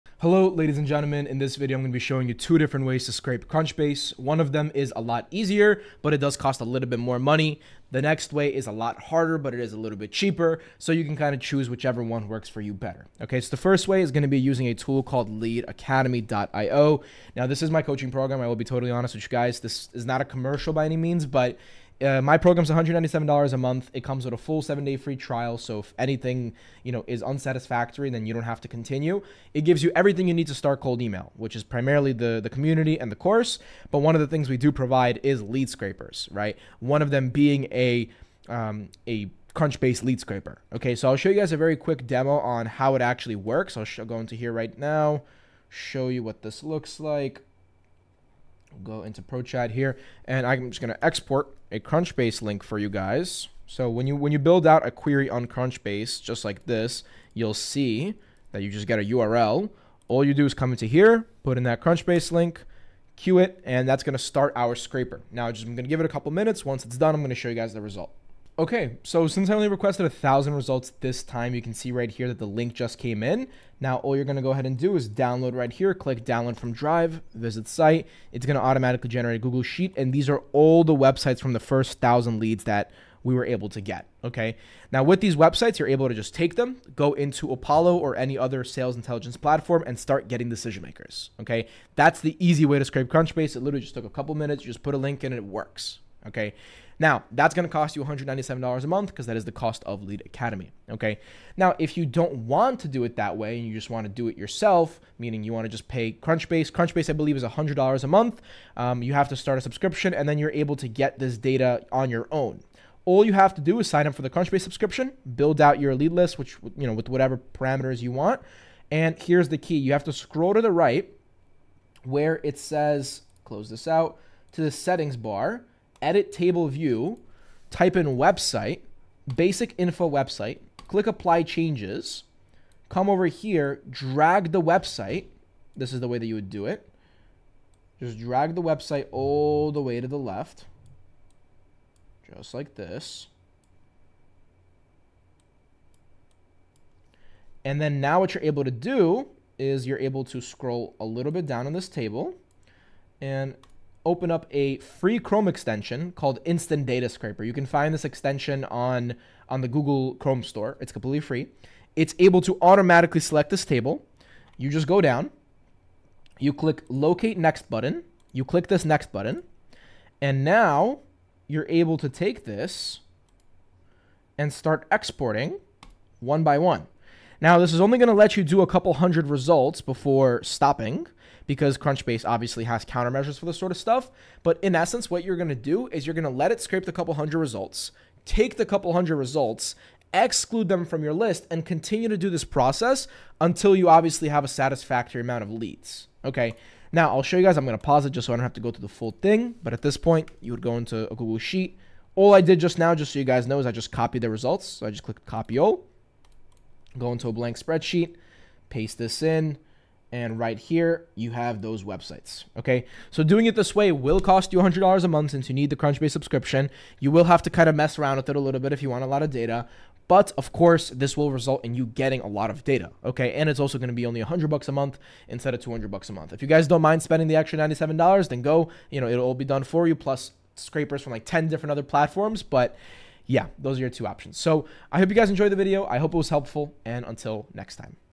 How To Scrape SaaS Leads Using Crunchbase for FREE (2025 UPDATE)
••👉 Join Lead Academy For 7-DAYS FREE?

🚀 How to Scrape Crunchbase for SaaS Leads in 2025 (FREE & Scalable Methods) 🚀

Want to find fast-growing SaaS companies without spending thousands on tools like ZoomInfo ?

In this video, we walk through step-by-step how to pull high-quality SaaS leads from Crunchbase—perfect for agencies, consultants, software companies, and anyone targeting funded B2B tech startups.

Whether you're building cold outreach campaigns, lead lists for investors, or targeting specific verticals like AI, marketing tech, or FinTech—this tutorial gives you the blueprint to extract valuable insights without breaking the bank.

🔍 What You'll Learn:
• How to scrape Crunchbase leads manually for free
• The best low-cost tools to scale your SaaS lead extraction
• How to filter by funding round, industry, employee size, and location
• Ways to enrich Crunchbase data with emails, LinkedIn URLs, and tech stack
• Advanced tips to identify ideal SaaS prospects for cold outreach
• How to organize your list for Smartlead, Instantly, Clay, or Close.io

🎯 Key Takeaways:
• Scrape thousands of SaaS companies without paying for Crunchbase Pro
• Find startups that just raised capital and are ready to buy
• Filter by ICP traits like industry, headcount, and region
• Build outbound campaigns with precision-targeted B2B data

If you’re serious about scaling your SaaS outreach and building lead lists that actually convert—this is the video for you.